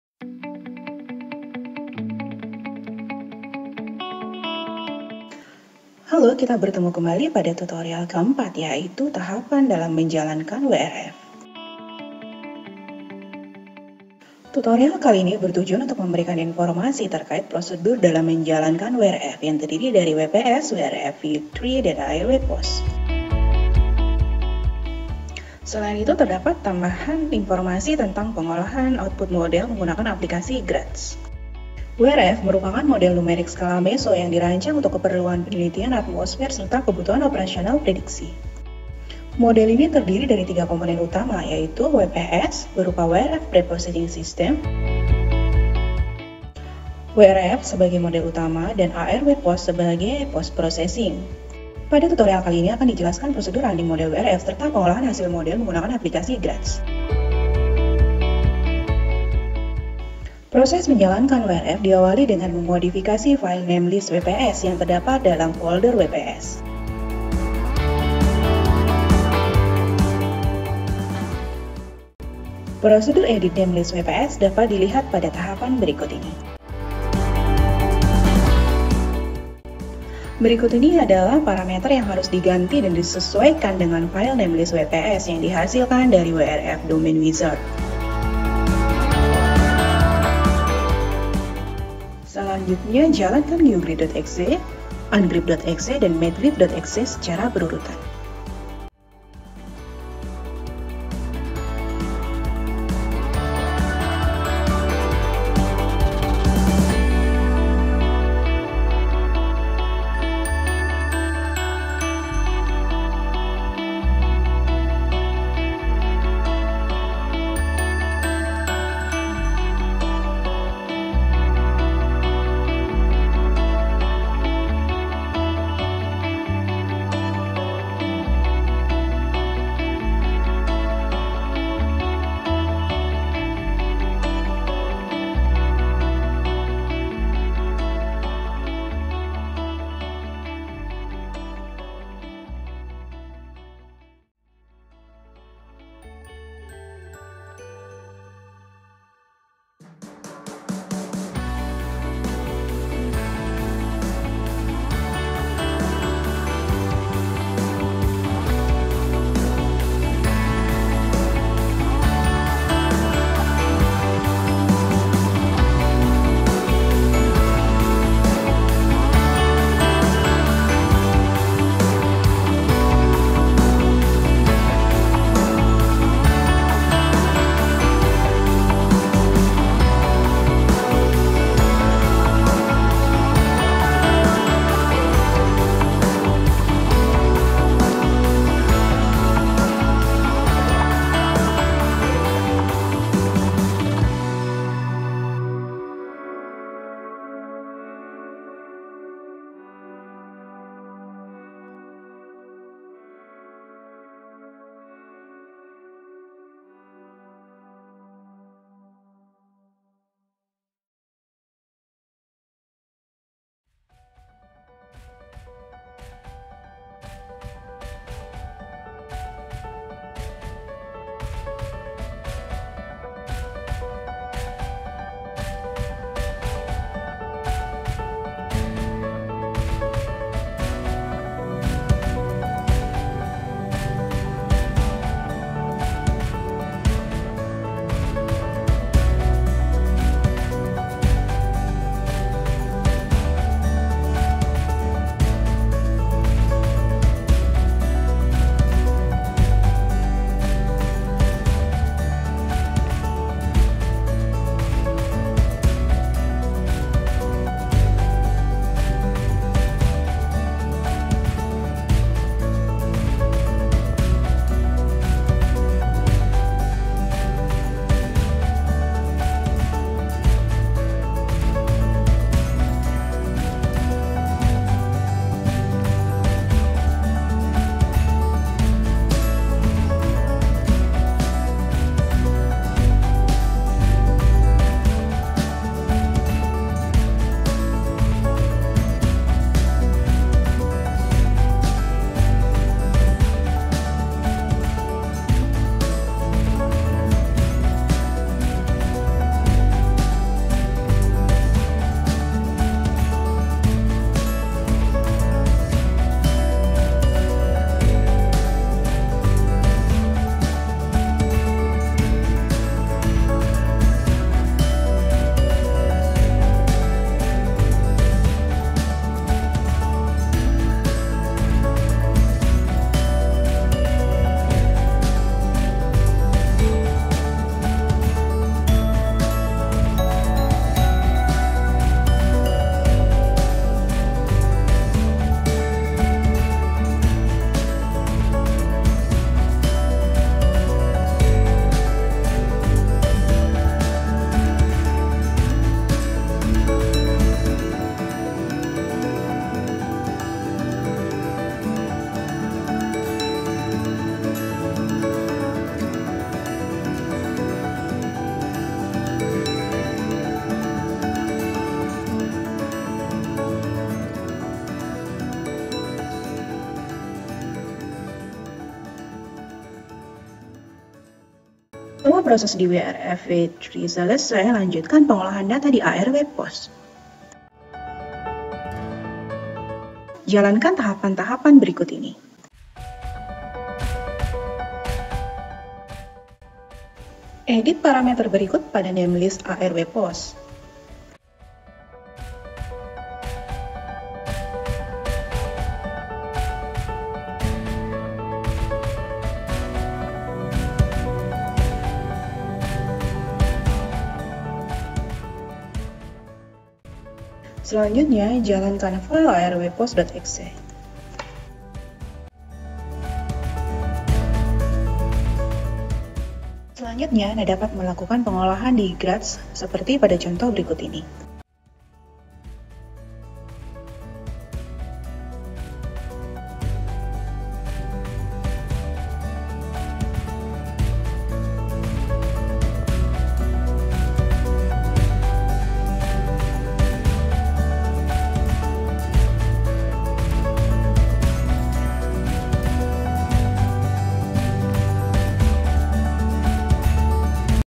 Tutorial 4 - Menjalankan WRF
Video ini merupakan bagian keempat dari seri Tutorial untuk Praktikum Kuliah AK3112 Dasar Pemodelan Cuaca dan Iklim. Video ini akan menjelaskan tahap menjalankan WRF.

Credit: Wirid Birastri M.Si., Alvin Pratama S.Si., MT

Ucapan terima kasih kepada: Dr. Nurjanna Joko T., Dr. M. Rais Abdillah, dan Dr. Moedji Raharto